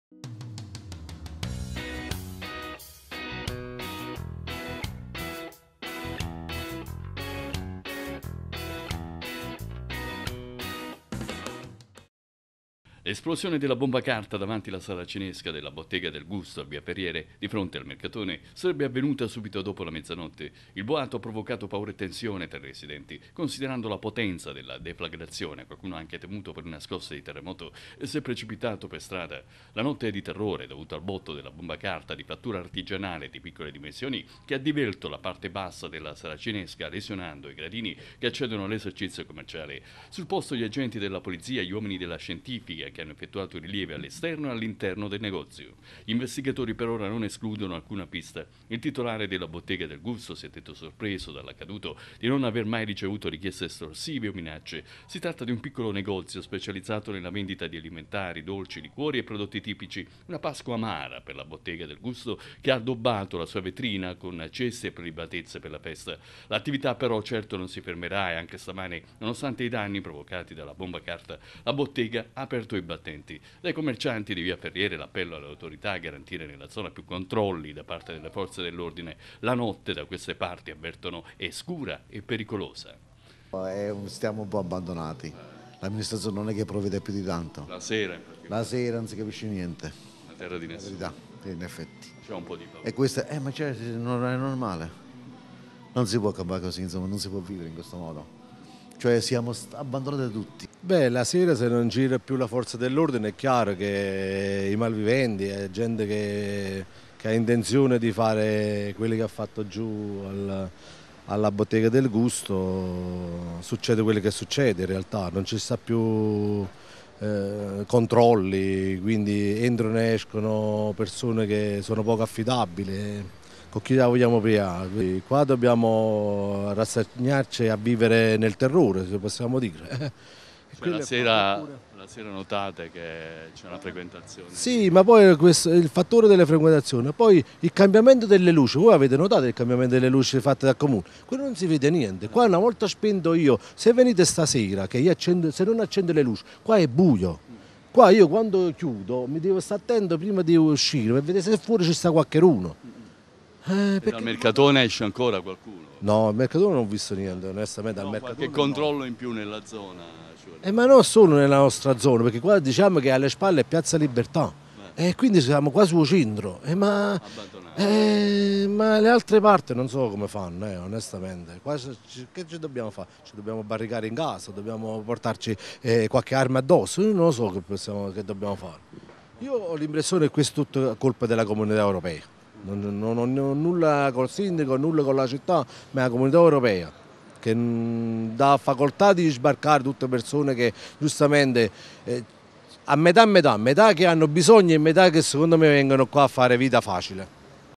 Nella notte bomba carta in via ferriere i residenti hanno paura
Boato nella notte in Via Ferriere ad Avellino. La bomba carta è esplosa davanti ad un negozio di alimentari. Sul caso indaga la polizia. I residenti chiedono maggiori controlli.